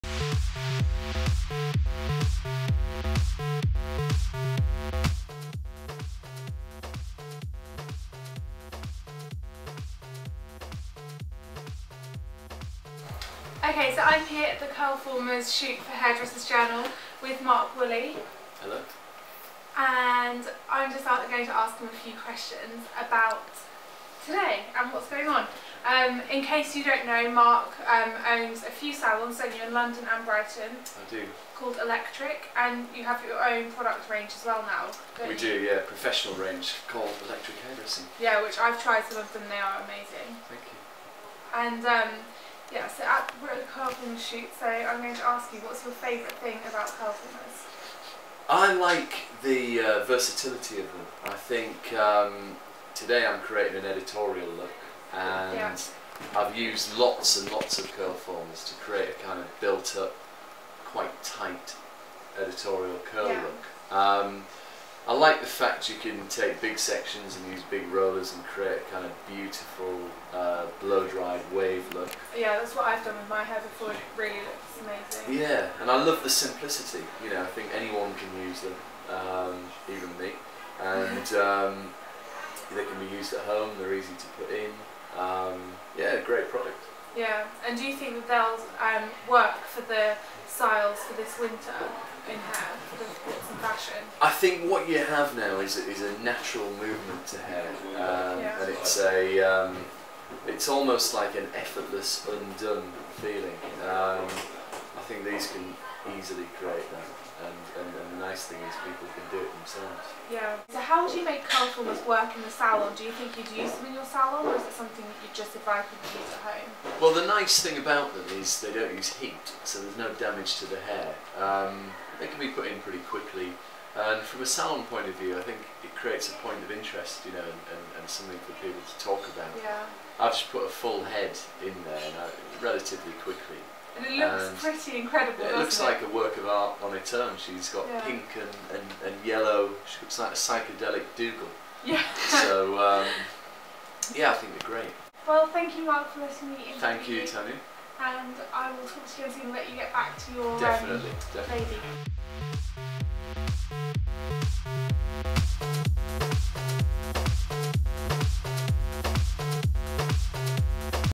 Mark Woolley interviewed by Tanya Burr at the Curlformers Hairdresser Journal photo-shoot
Mark Woolley, founder and director of Electric Hairdressing is interviewed by hair and beauty vlogger Tanya Burr on the set of the Curlformers Hairdresser Journal photo-shoot.

British Hairdressing Business Awards 2010  Director of the Year Hair Magazine 2009  Salon Stylist of the Year

Salons in Brighton, London and Reading.  Works with haircare brands Electric, LOreal and Denman.